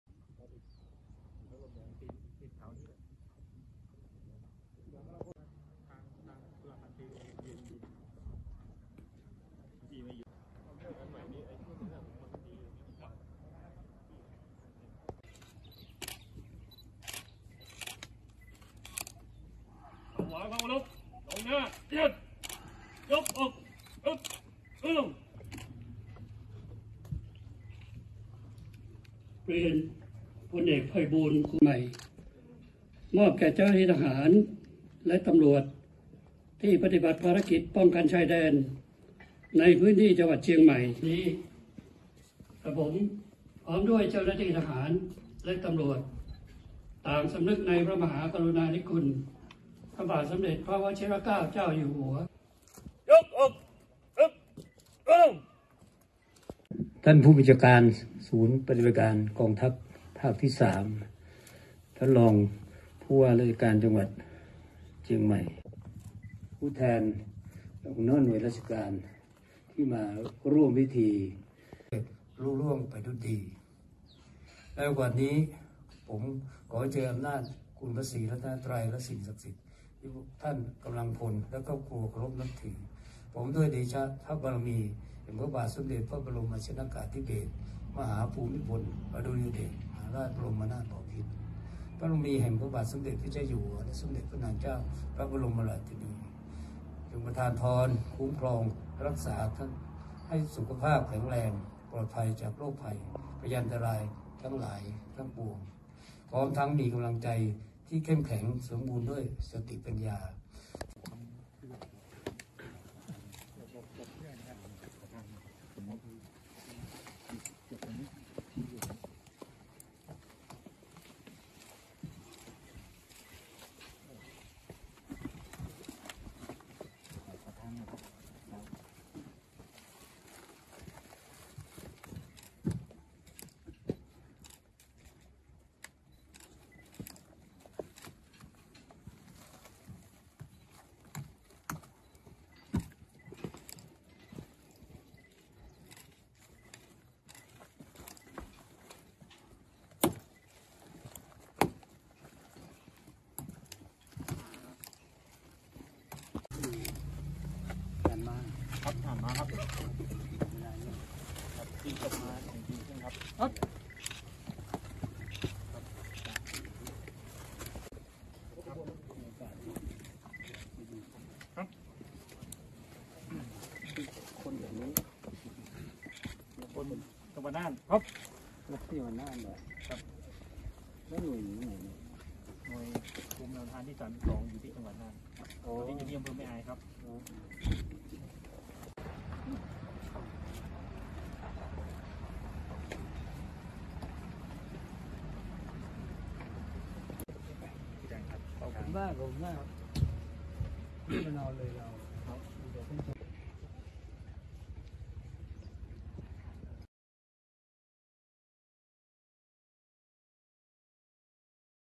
องคมนตรี เชิญสิ่งของพระราชทาน มอบเจ้าหน้าที่ฯ ชายแดน
26 ธันวาคม 2565 พล.อ.ไพบูลย์ คุ้มฉายา องคมนตรี เชิญสิ่งของพระราชทาน เนื่องในโอกาสปีใหม่ 2566 นำมอบกำลังพล